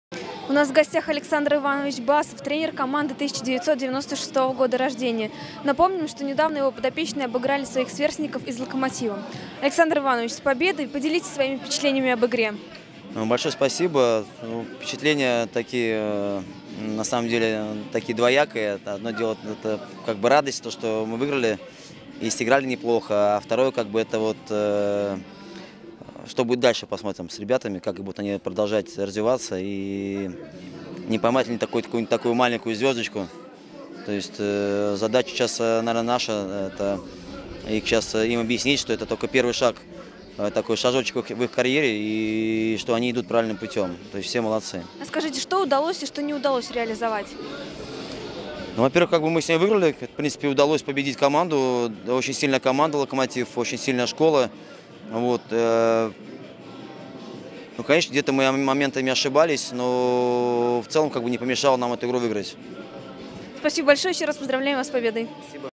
Интервью Александра Басова после Локо-Строгино 13.04.13
Интервью Александра Басова после победного матча U-17 Локо - Строгино 2:3.
13 апреля 2013 г.